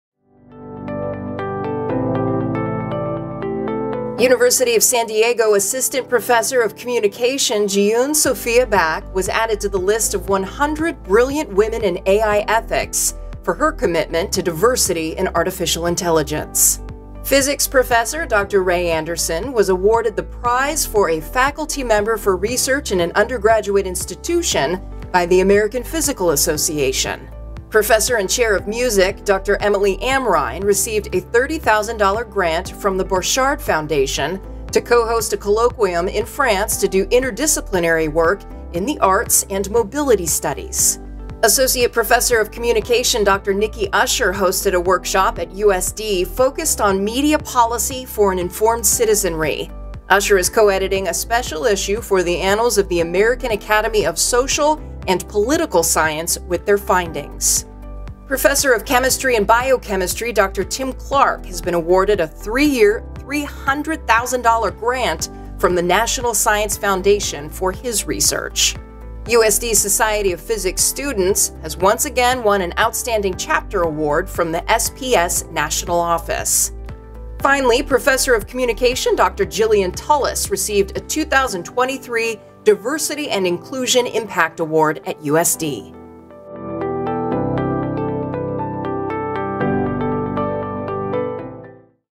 USD College of Arts and Sciences: Faculty and Student Accomplishments
Transcript:

University of San Diego Assistant Professor of Communication Jeeyun (Sophia) Baik was added to the list of 100 Brilliant Women in AI Ethics for her commitment to diversity in AI.

Physics Professor Dr. Rae Robertson-Anderson was awarded the Prize for a Faculty Member for Research in an Undergraduate Institution by the American Physical Association.

Professor and Chair of Music Dr. Emilie Amrein received a $35,000 grant from the Borchard Foundation to cohost a colloquium in France to do interdisciplinary work in the arts and mobility studies.

Associate Professor of Communication Dr. Nikki Usher hosted a workshop at USD focused on Media Policy for an Informed Citizenry. Usher is co-editing a special issue for the ANNALS of the American Academy of Social and Political Science with their findings.

Professor of Chemistry and Biochemistry Dr. Tim Clark has been awarded a three-year, $300,000 grant from the National Science Foundation for his research.

USD’s Society of Physics Students has once again won an Outstanding Chapter Award from the SPS national office.

Finally, Professor of Communication Dr. Jillian Tullis received a 2023 Diversity and Inclusion Impact Award at USD.